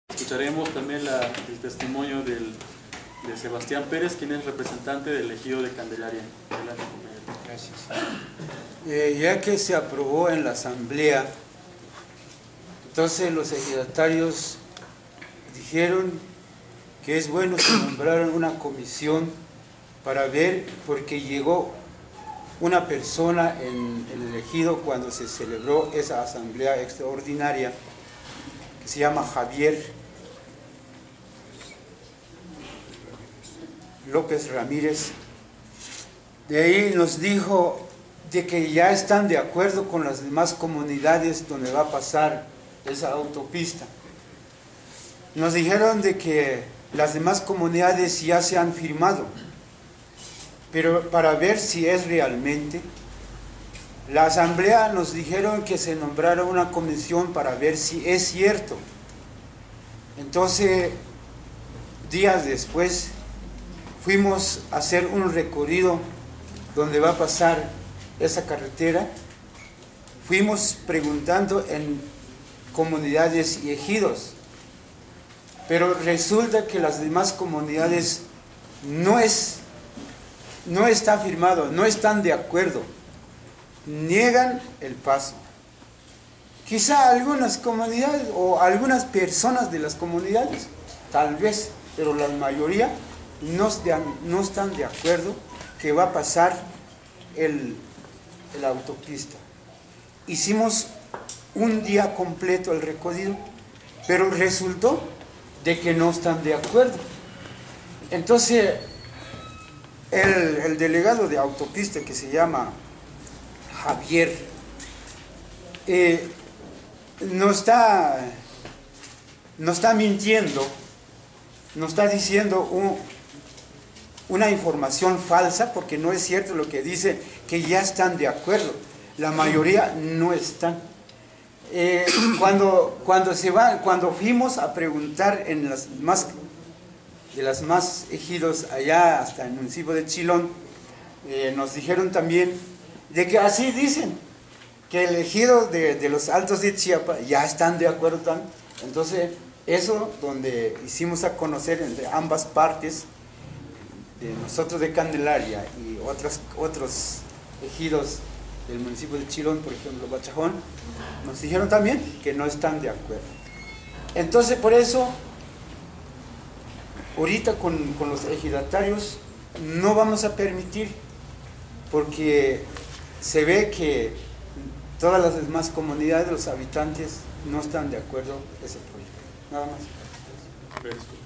Representante Ejido La Candelaria, SCLC - No a la autopista San Cristóbal - Palenque
Habitantes de La Candelaria, de San Cristóbal de Las Casas,  invitan a que se sumen para detener la construcción de la autopista San Cristóbal - Palenque.

El argumento más importante es que no están consultando ni respetando la voluntad de las comunidades originarias.